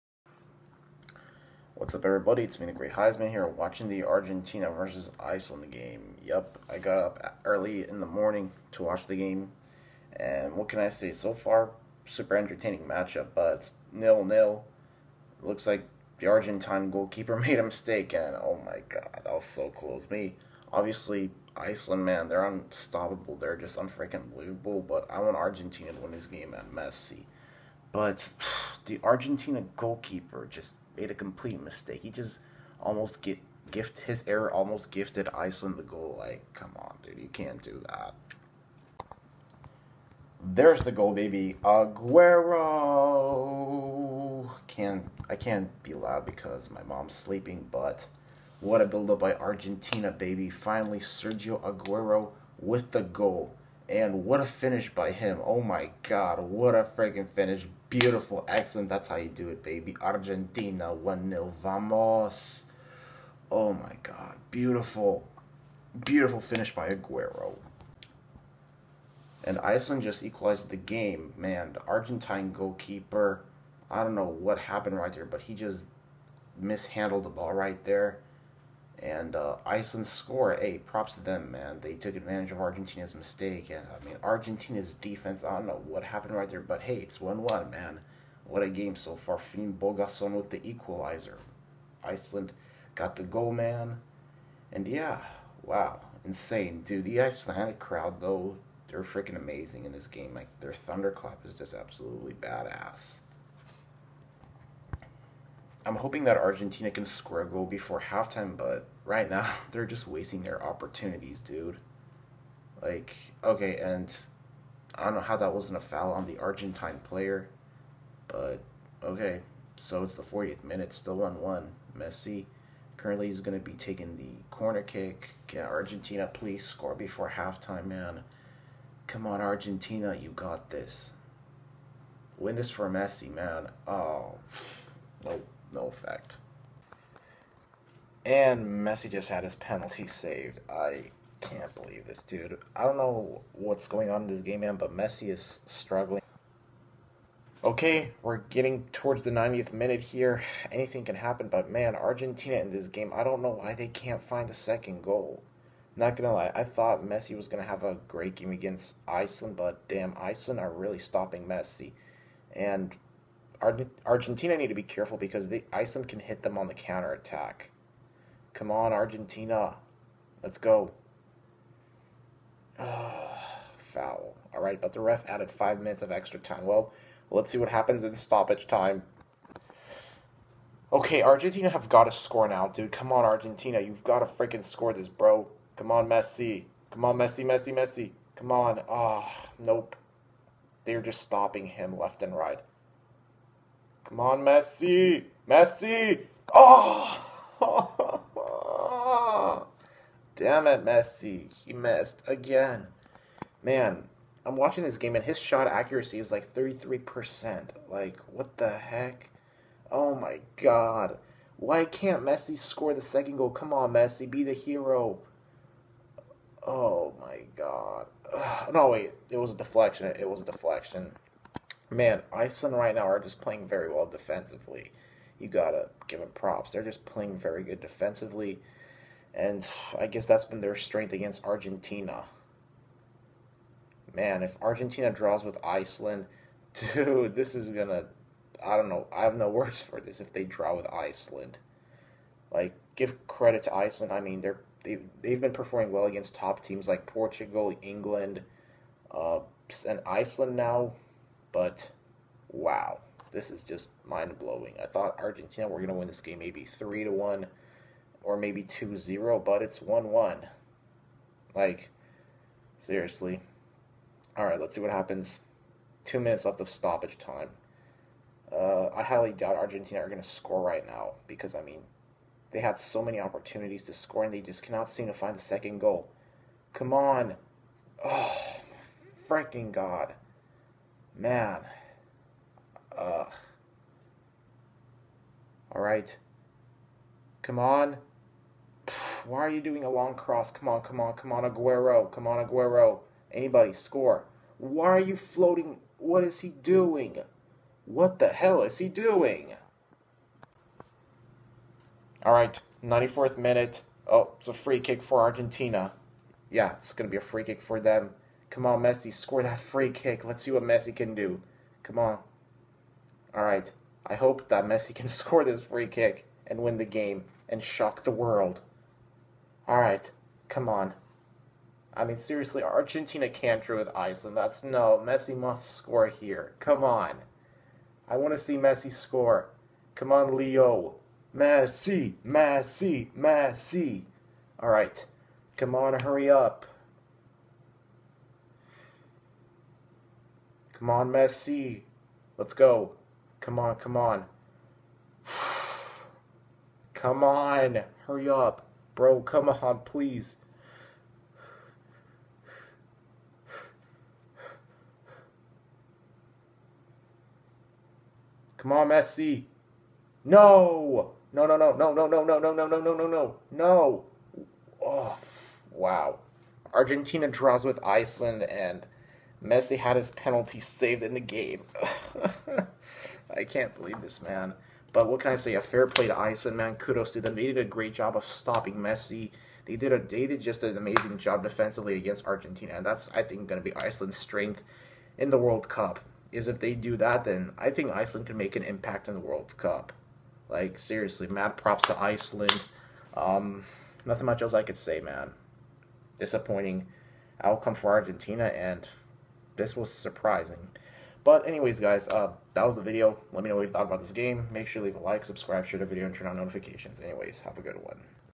Argentina vs Iceland(1-1) 2018 FIFA World Cup + Messi Penalty Save Reaction!
Argentina surprisingly draw 1-1 with Iceland and Messi had his penalty saved.